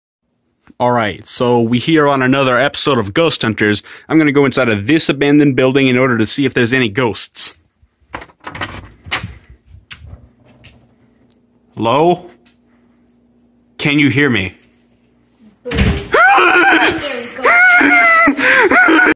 Ghost Hunters in a Nutshell
Ghosts appear to be haunting my house, I would say I'm happy to have the geese away but I get the impression they're responsible for this.